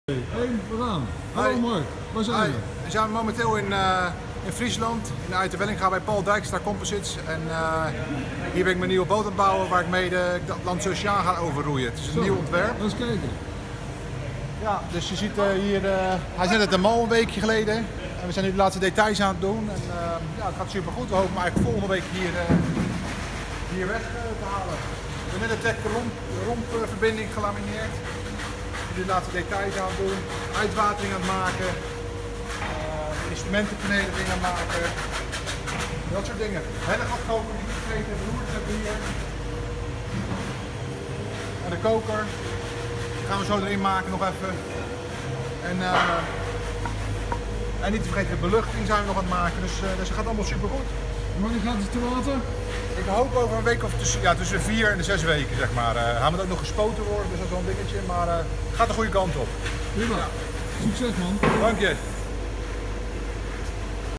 Nieuwe boot Mark Slats
Mei 2020 - Oceaanroeier Mark Slats bouwt momenteel in Uitwelleringa in de loods van Paul Dijkstra Composites aan zijn nieuwe oceaan roeiboot.
Jachtontwerper Dick Koopmans tekende voor het ontwerp. Door de combinatie van carbon composiet en voorgevormd schuim is de boot ultralicht en sterk. In Roei! 38 van juni 2020 staat het hele verhaal met foto’s. Video: Jos Wassink